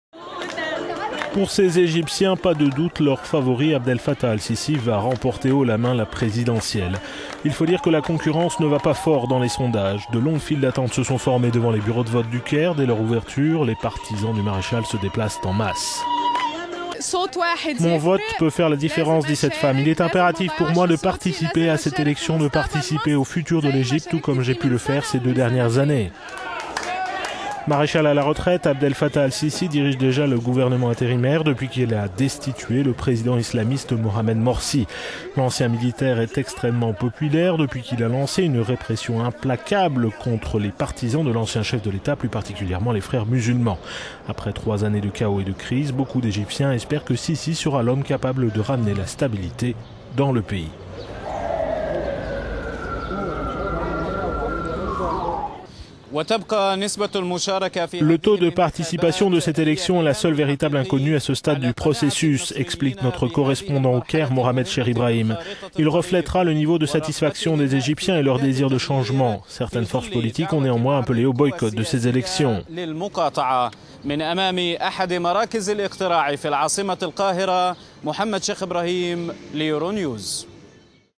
Présidentielle egyptienne, Abdel Fattah al-Sissi donné largement favori
Pour ces Egyptiens, pas de doute leur favori Abdel Fattah al-Sissi va remporter haut-la-main la présidentielle. Il faut dire que la concurrence ne va pas fort dans les sondages.
De longues files d'attente se sont formées devant les bureaux de vote du Caire dès leur ouverture. Les partisans du maréchal se déplacent en masse...

"Mon vote peut faire la différence. Il est impératif pour moi de participer à cette élection, de participer au futur de l'Egypte, tout comme j'ai pu le faire ces deux dern...